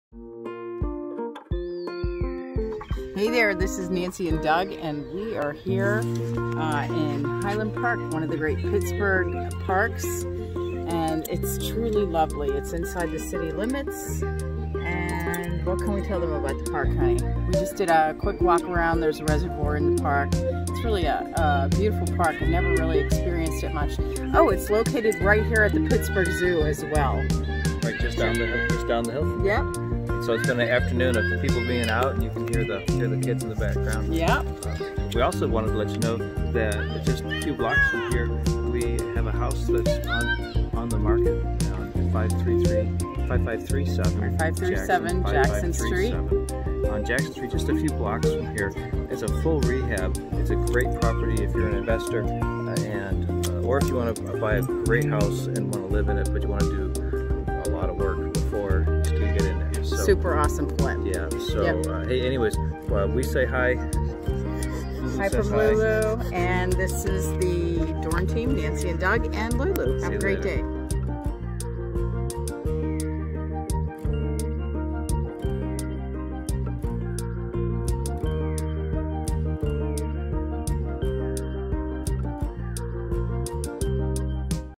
Highland Park with Lulu
Pittsburgh's city parks are often forgotten as a gigantic advantage to living in the city. Today, Nancy and Doug explore the benefits of Highland Park in Pittsburgh's east end.